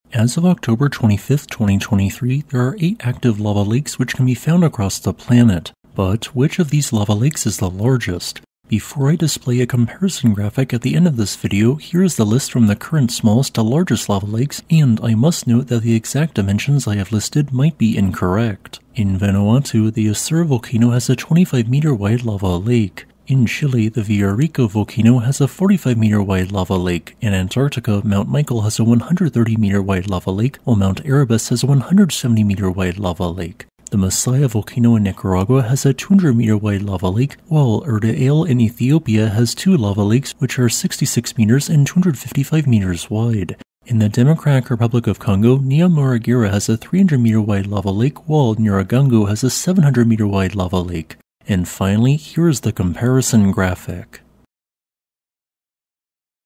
Which Volcano Has the Largest Active Lava Lake?
There are currently 8 active lava lakes which can be found at volcanoes across 5 continents. But, which of these lava lakes is the largest? This video discusses the specifics. NOTE: The details in this video were only up to date as of October 25th 2023. Also, the dimensions of these active lava lakes, or suggestion that they are currently present (as of October 25th 2023) might be incorrect as presented in this video.